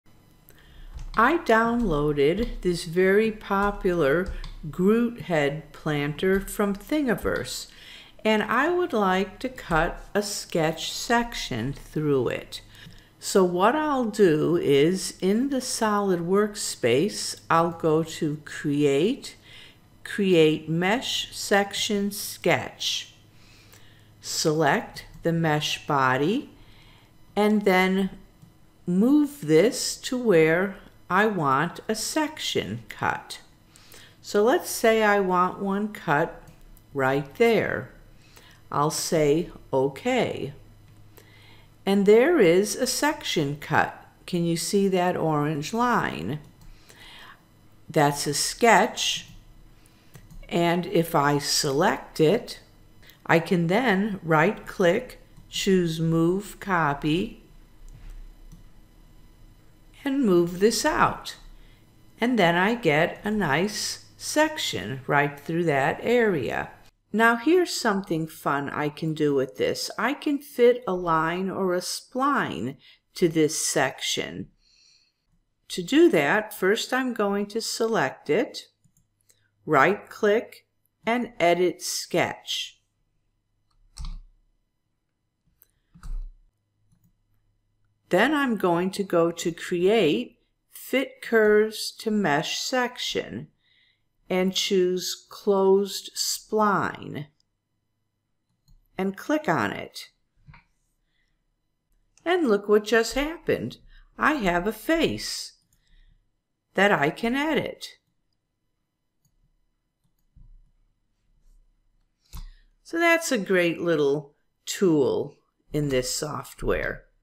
Fusion 360: Fit Curves to Mesh Section for Reverse Engineering
Make a section through a mesh model and then fit a spline inside it to make a face. Useful for reverse engineering.